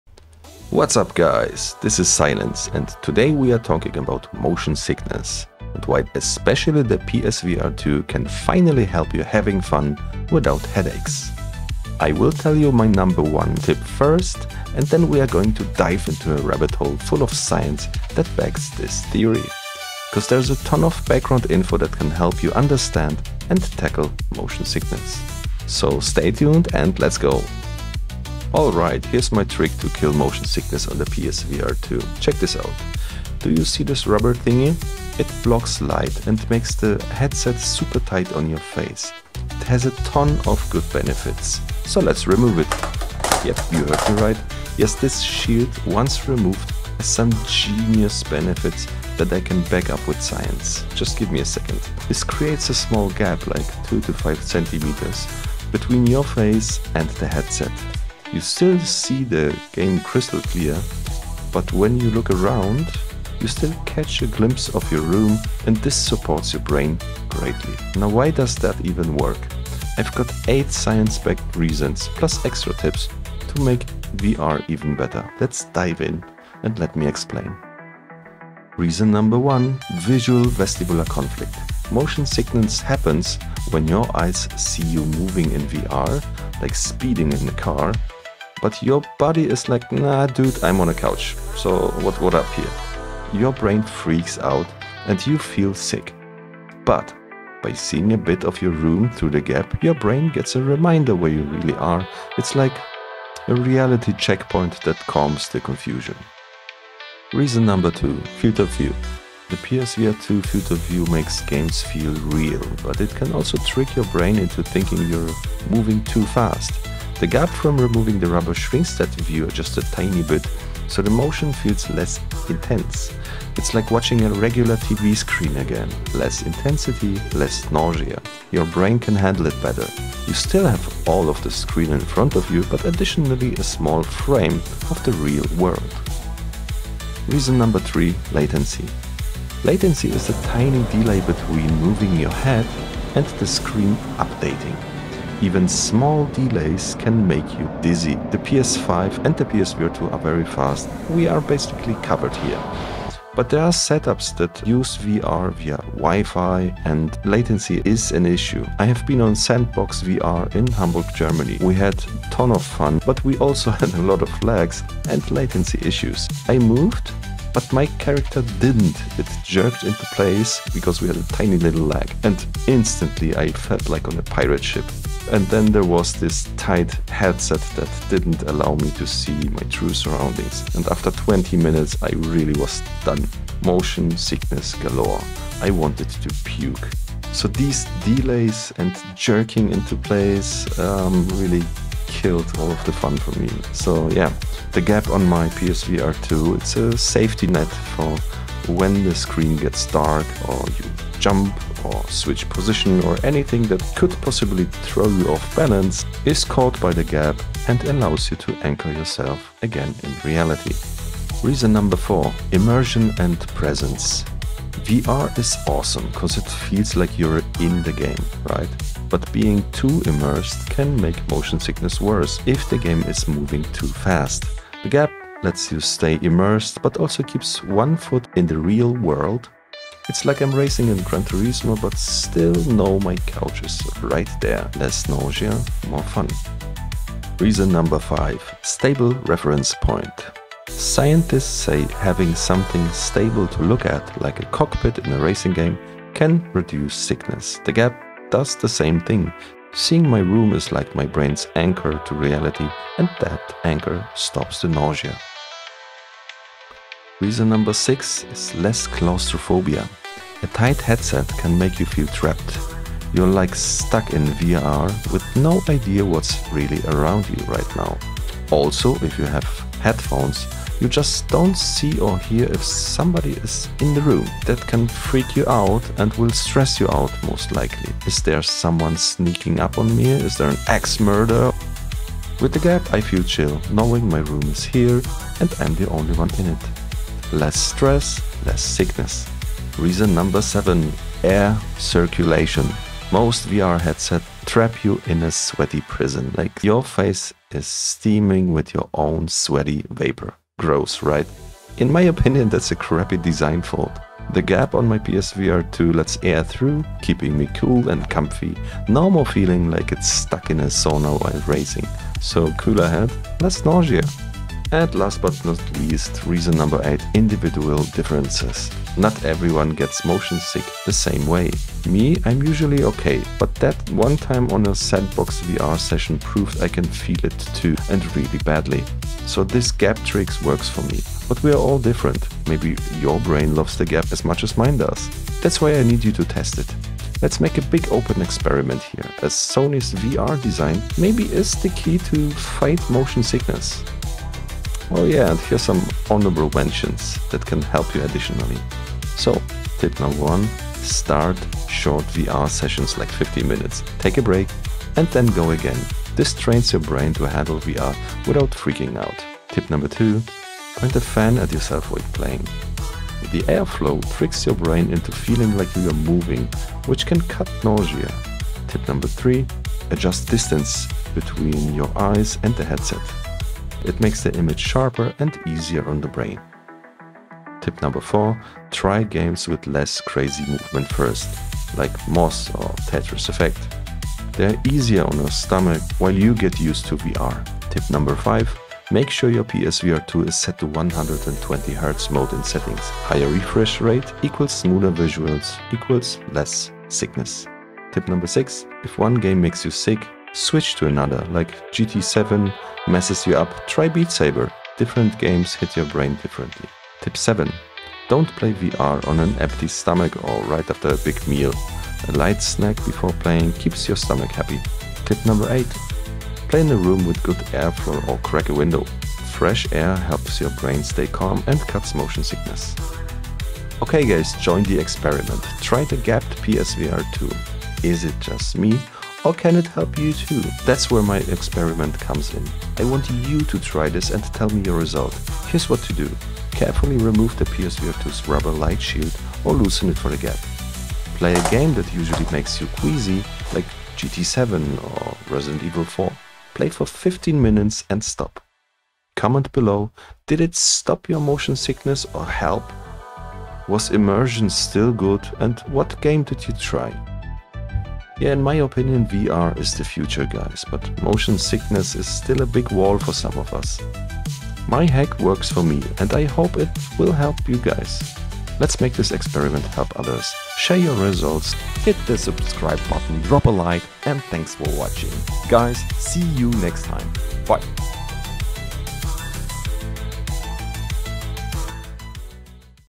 No more VR Sickness with This PSVR2 Trick!
Join the Experiment! I’m SciLens, your guide to VR !   - Motion sickness can ruin your PSVR2 fun, but I’ve got a game-changing trick that pushes the boundrys of the PSVR2 Design.
It helps a lot with stop nausea! In this video, I show you how to do it, share 8 science-backed reasons why it works in theory, and drop extra tips to keep your VR sessions smooth.

Join my SciLens Experiment! Try the hack and comment:
Did it stop your motion sickness?
How was immersion?
What game did you play (e.g., Gran Turismo 7, Beat Saber)?